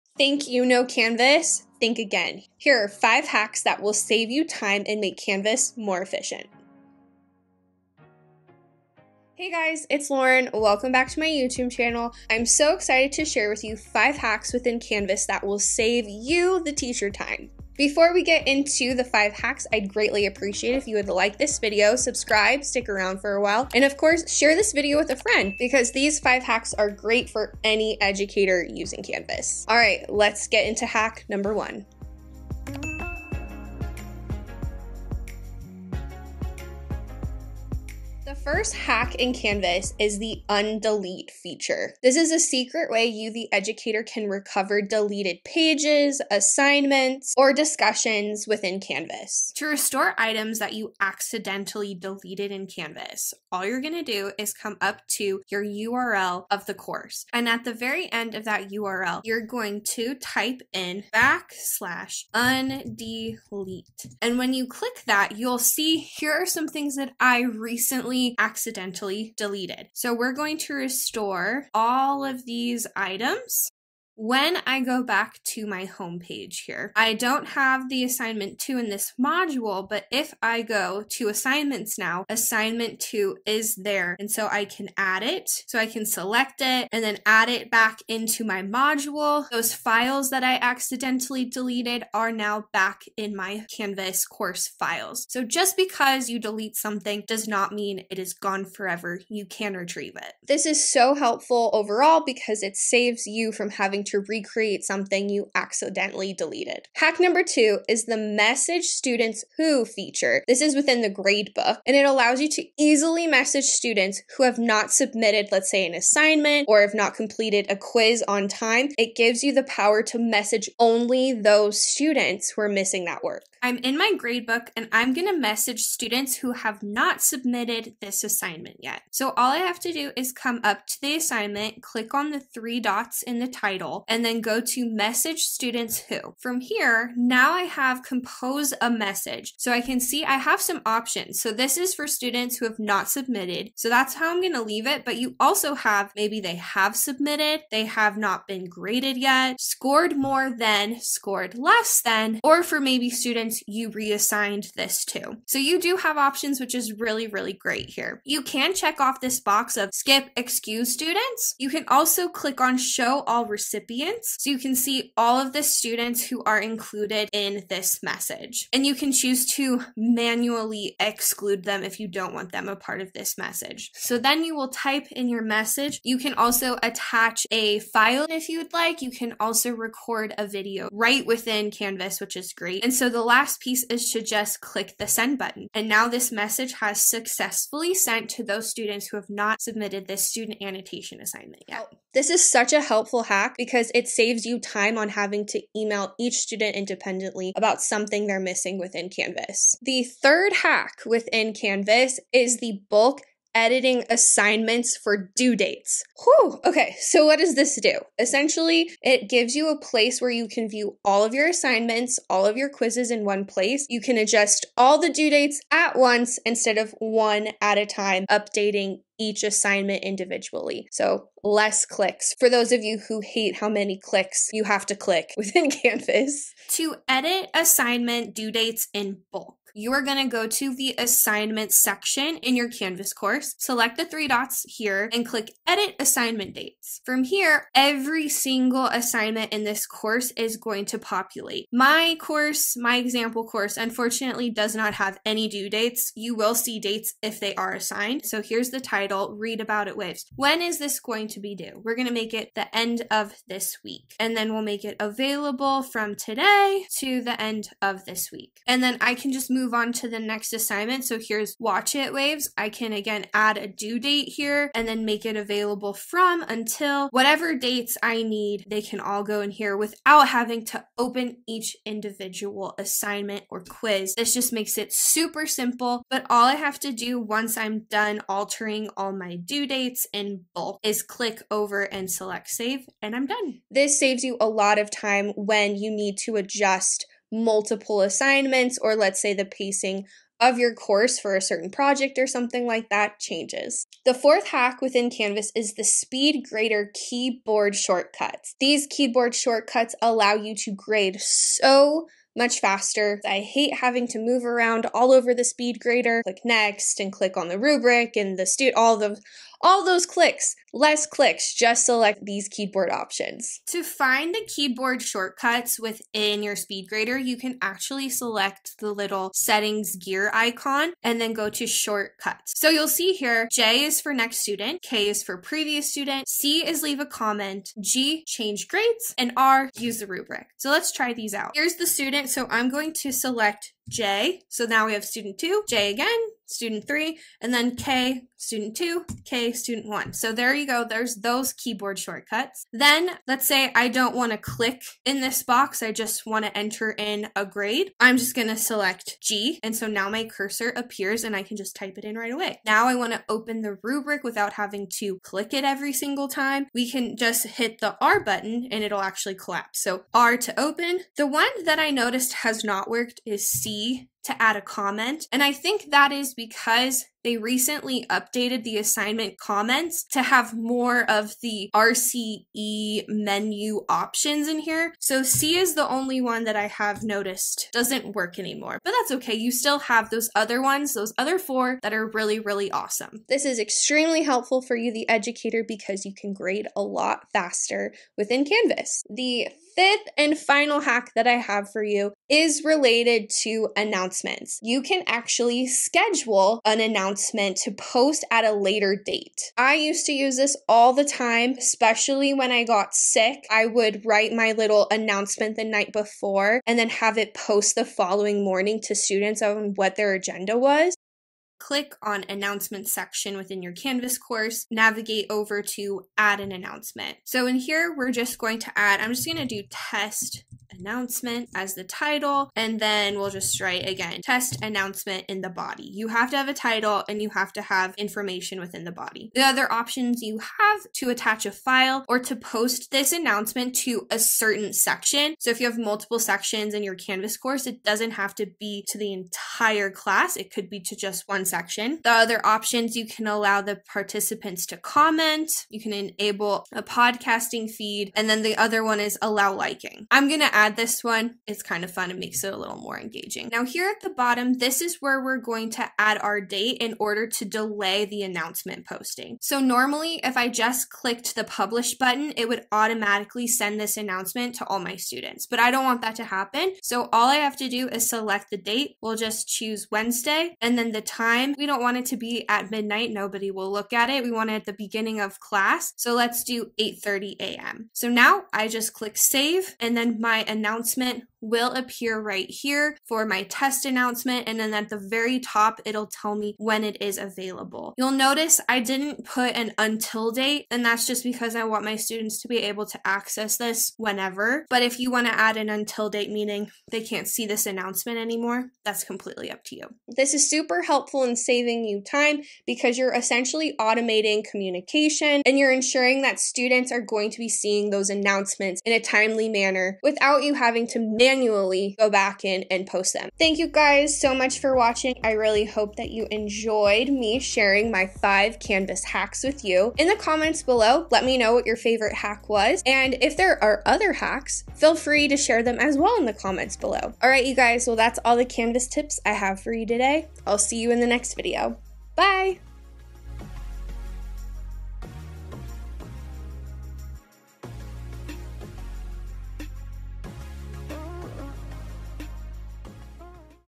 🚀 5 Canvas LMS Hacks You Wish You Knew Sooner! 🎓
Think you know Canvas LMS? Think again! These 5 secret hacks will save you time, boost efficiency, and help you engage students like never before! 💡 Whether you're a teacher, admin, or course designer, these Canvas LMS tips and tricks will level up your workflow instantly.

🎯 Why These Hacks Matter:
✅ Recover lost work in seconds
✅ Communicate with students effortlessly
✅ Save hours on grading & assignments
✅ Automate your announcements

💬 What’s your favorite hack? Do you have a Canvas LMS tip to share? Drop it in the comments below! 👇

📚 Chapters:
00:00 Introduction
00:38 Hack #1: Undelete Trick
01:46 Hack #2: Message Students Who...
03:19 Hack #3: Bulk Edit Assignments
04:53 Hack #4: Keyboard Shortcuts
06:28 Hack #5: Schedule Announcements
08:33 Outro: Which Hack Was Your Favorite?

👍 Like this video if you found it useful!